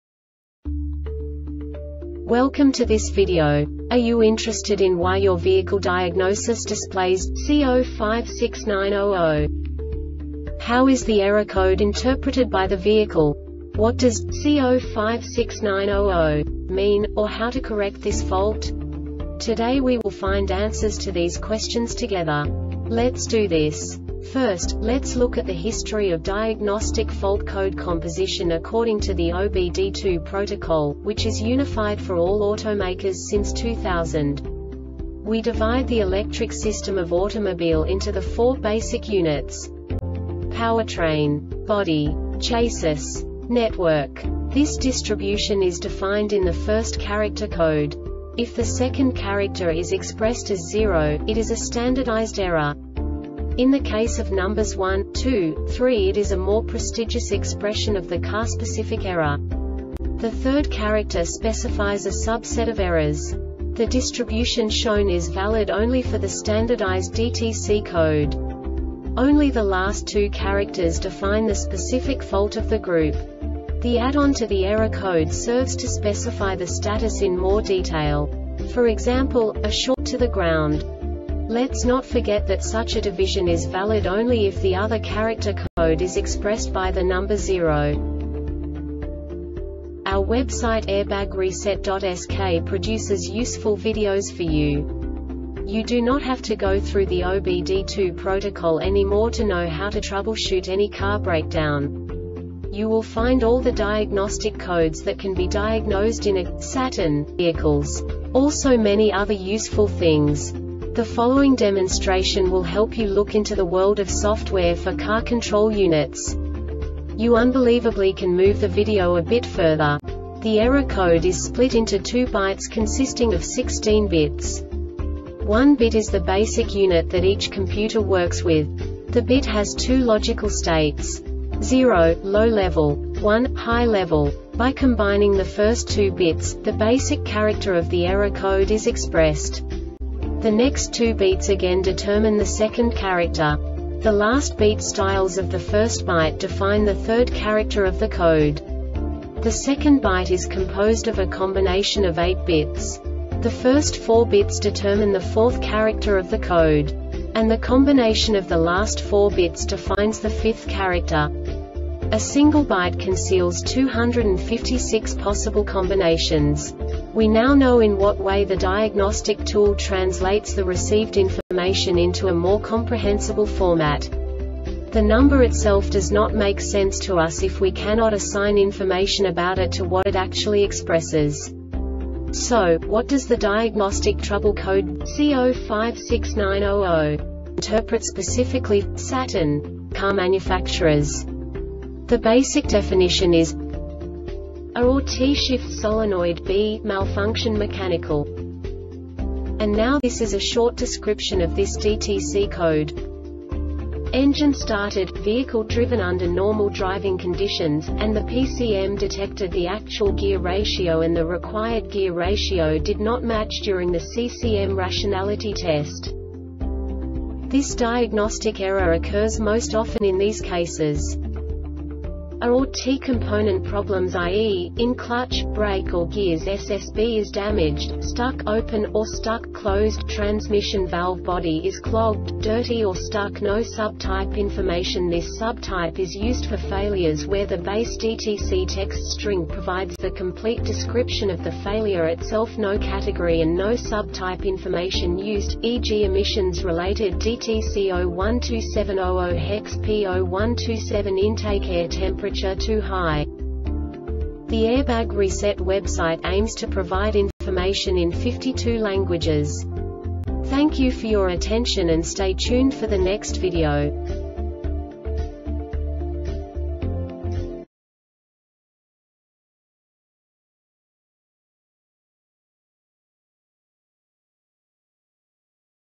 DTC Saturn C0569-00 Short Explanation
Contents:
0:21 Basic DTC analysis according to OBD2 protocol standard.
1:48 Insight into programming
2:58 A diagnostically understandable description of the Diagnostic Trouble Code
3:05 Basic error definition

Make:
Saturn
Code:
C0569 00
Definition:
A or T Shift Solenoid B Malfunction (Mechanical)
Description:
Engine started,vehicle driven under normal driving conditions,and the PCM detected the Actual gear ratio and the Required gear ratio did not match during the CCM Rationality test.
Cause:
A or T component problems (i. e. ,in clutch,brake or gears)SSB is damaged,stuck "open" or stuck "closed"Transmission valve body is clogged,dirty or stuck
Failure Type: